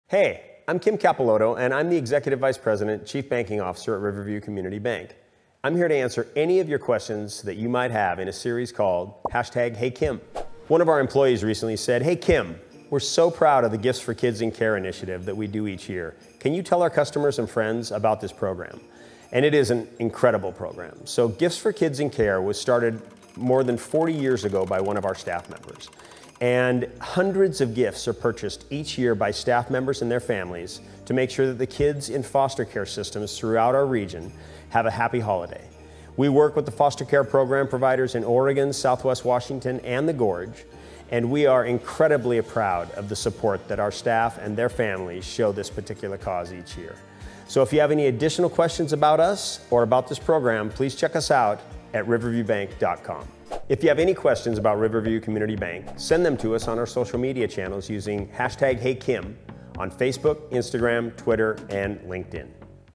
HeyKim, we're proud of Gifts for Kids. Can you tell our customers and friends about this program?
Ask Executive Vice President, Kim Capeloto all of your questions on social media, by using HeyKim​.

This week's question is from a Riverview staff member. HeyKim, we're proud of Gifts for Kids. Can you tell our customers and friends about this program?

Member FDIC and Equal Housing Lender